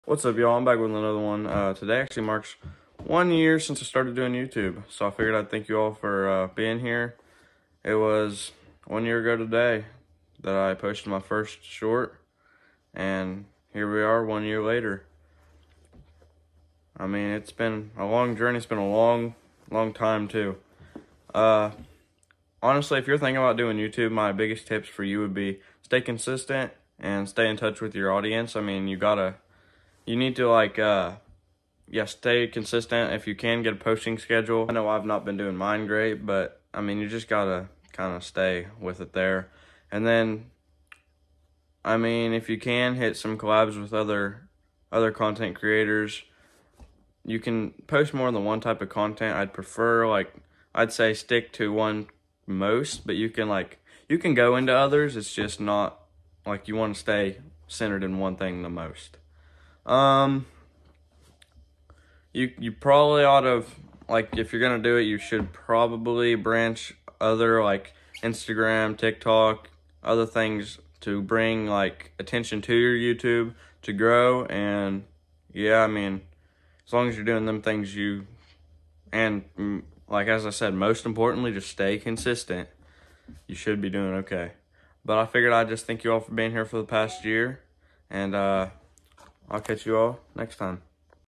ONE YEAR OF YOUTUBE- One year of doing YouTube…
Today marks one year of me doing YouTube. Through that time there’s been many ups and downs. Many lessons learned and many friends made. I hope you’ve enjoyed watching me through this time and I hope you enjoy wants to come.